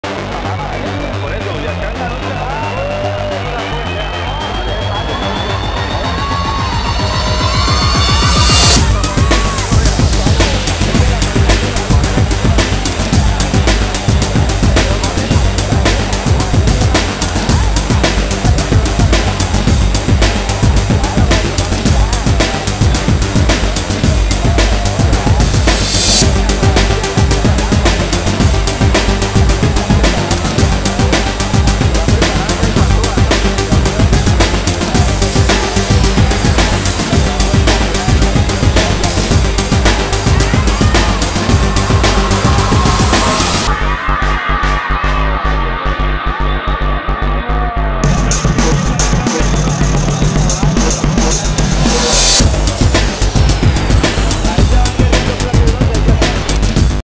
Asi se vivió en Buga las emociones del campeonato nacional de patinaje interligas 2016 - 2a. parte
Las noticias del mundo a tu alcance